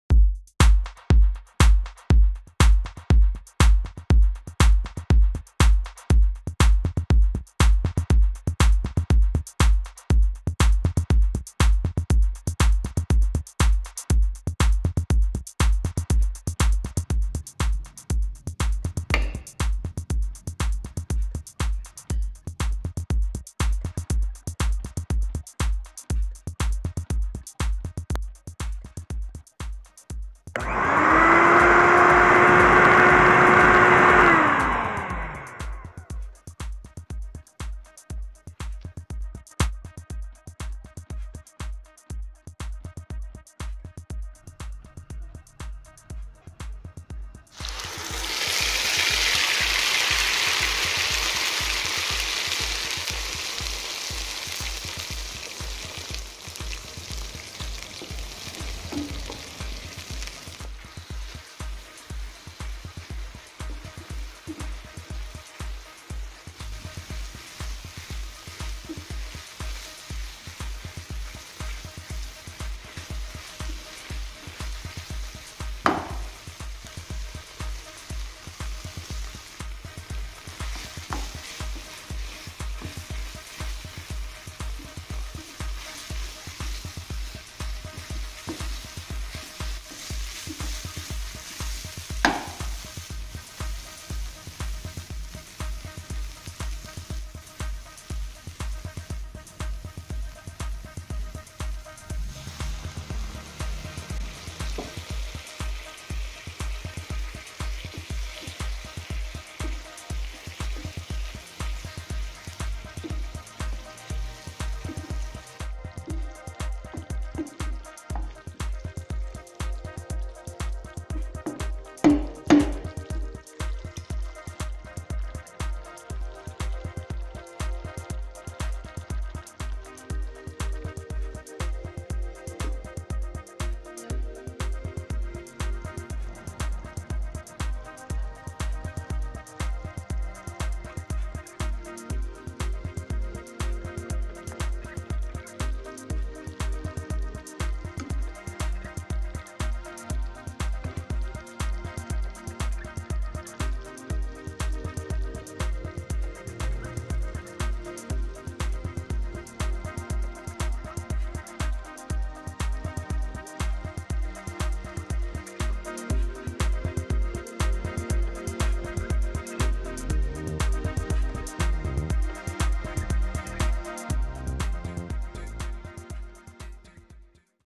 Malai Egg Curry | Egg Masala | Egg Gravy
In this video we will see how to Malai Egg Curry video.

Friends please do try this style Malai Egg Curry  recipe do share your feedback about the recipe in the comments below.